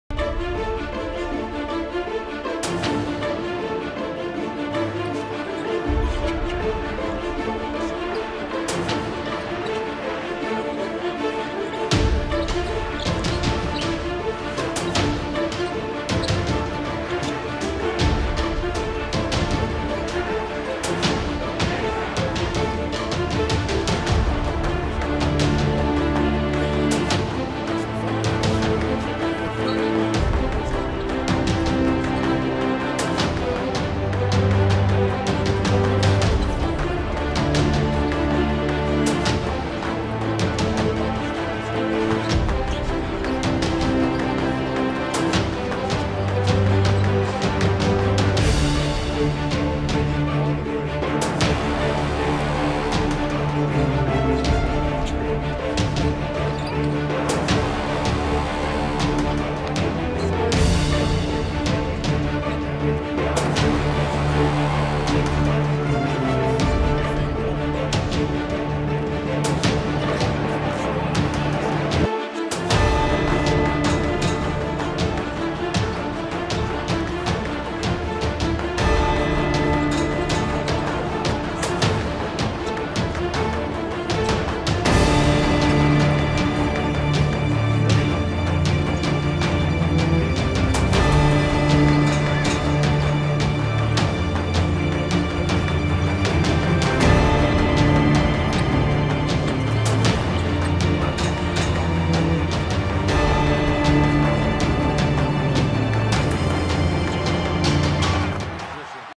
LOS ANGELES CLIPPERS VS SAN ANTONIO SPURS GAME 1 HIGHLIGHTS 4-19-2015
Los Angeles Clippers vs San Antonio Spurs Highlights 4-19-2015
Game 1 2015 NBA Playoffs Western Conference
Los Angeles W107-92